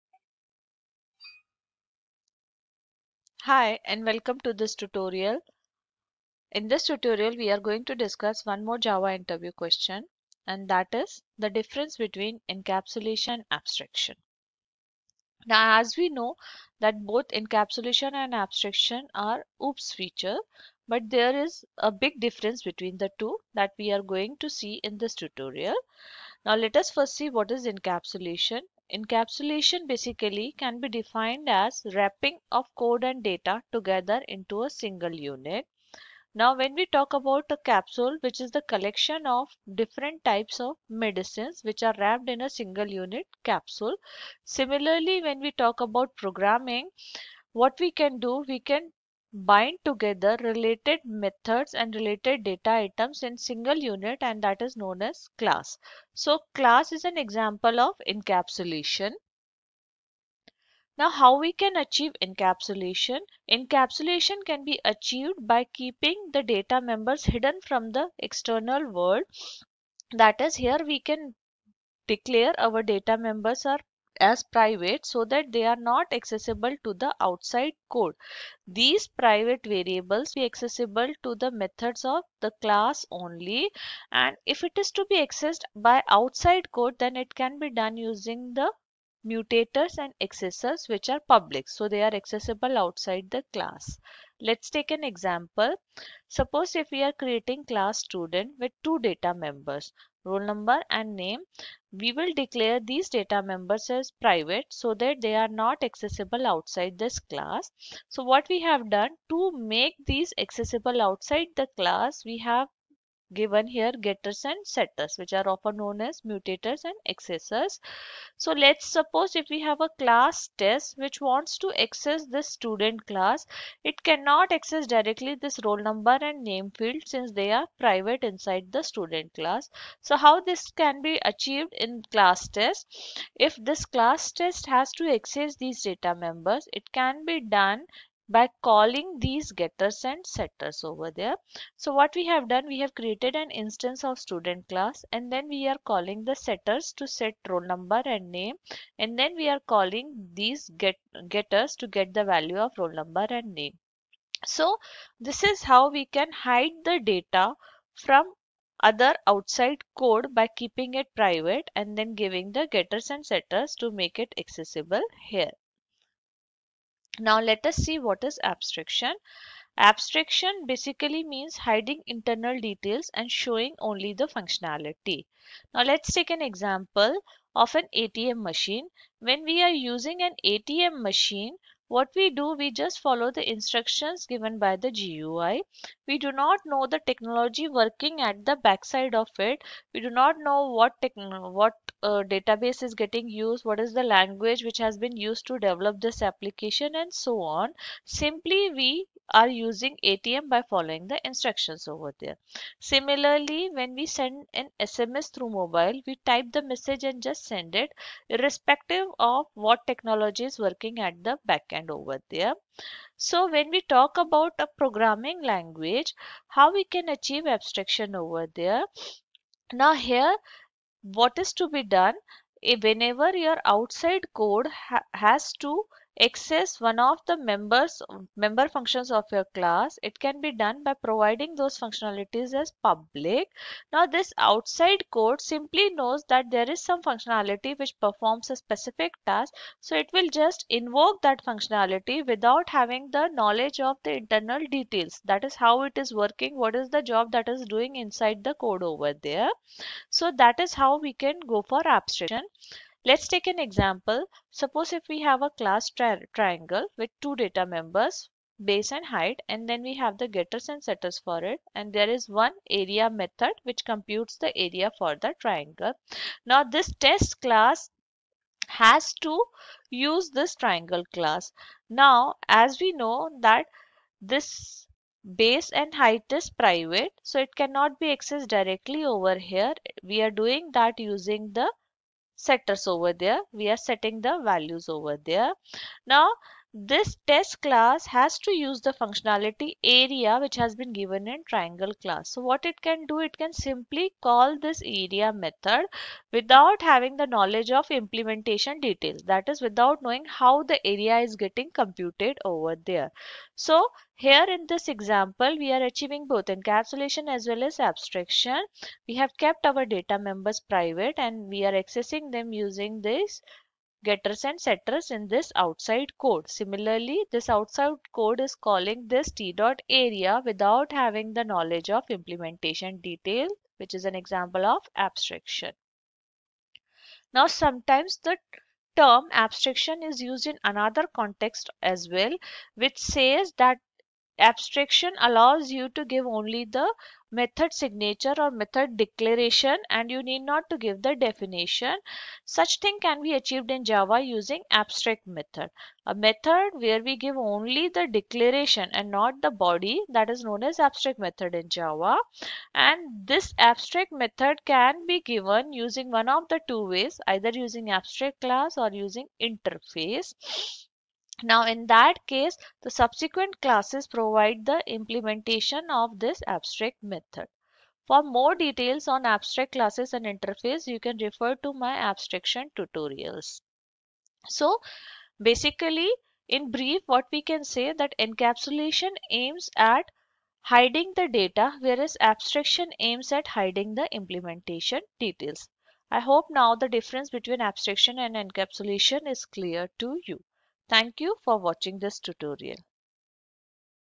Java Interview 05 - Encapsulation Vs Abstraction
This video explains the difference between encapsulation and abstraction.